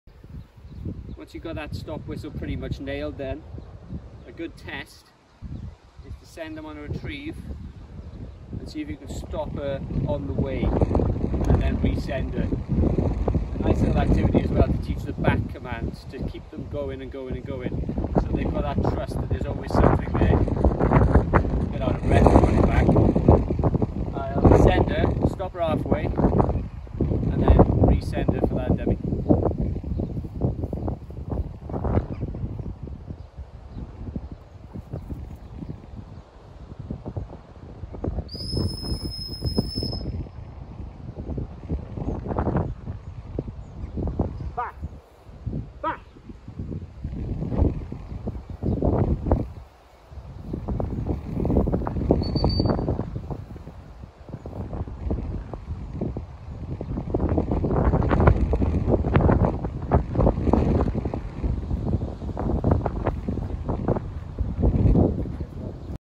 German Shorthaired Pointer (GSP) - Training- Stop Whistle Training - Exercise 2
The second exercise using the stop whistle can be more difficult as you are stopping the dog while on a retrieve. This can be important in the field especially if you have sent your dog as someone else does. The importance to have control is second to none.